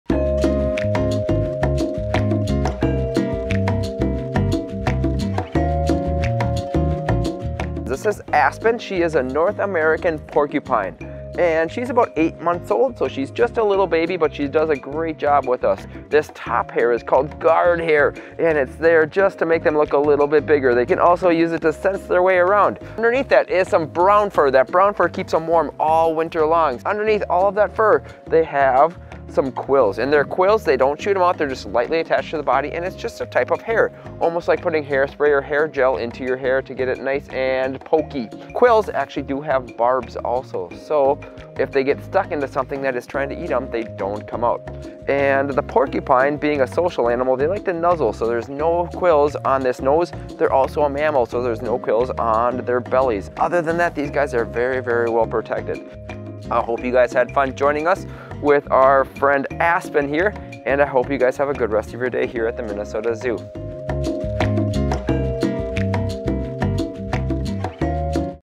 Minnesota Zoo Keeper Chat: North American Porcupine
Naturalist Donnie introduces us to Aspen, a North American porcupine about eight months old, and shares fun facts about how her hair, fur, and quills protect her against Minnesota winters and would guard against predators in the wild!